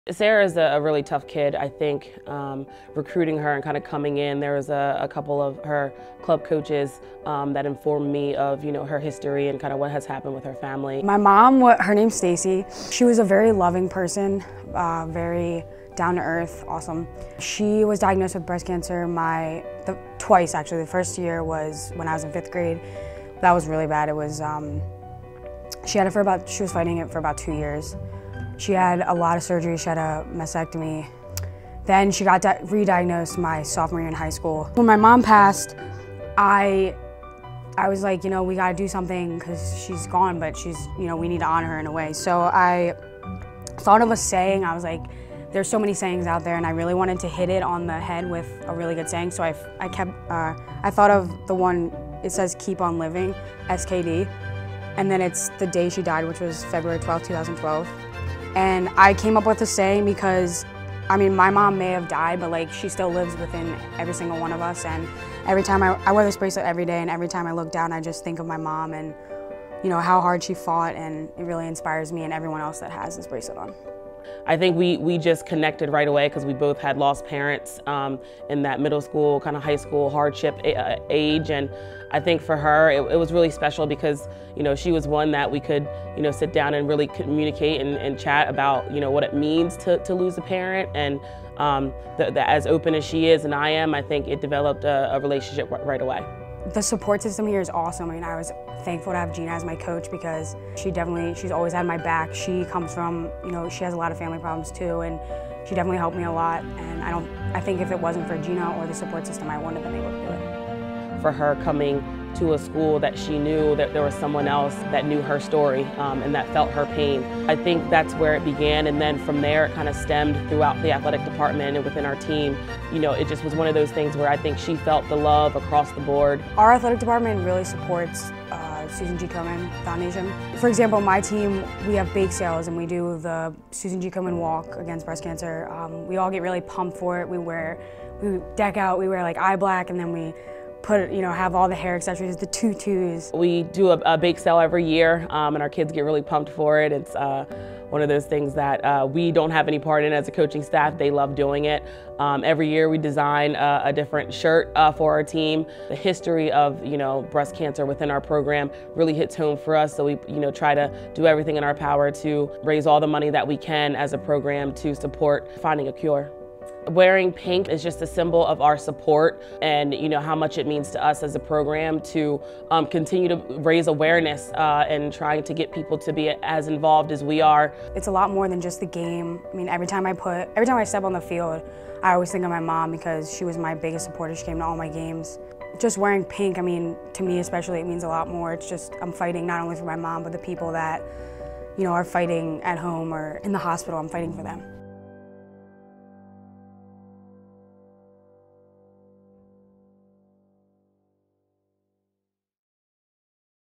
Cincinnati Lacrosse: Sarah Del Bene's Story
Bearcats lacrosse senior Sarah Del Bene lost her mother to breast cancer right before coming to Cincinnati to play for head coach Gina Oliver. Here she tells the story of her mother's fight and what it means for her to have the support from the Lacrosse team.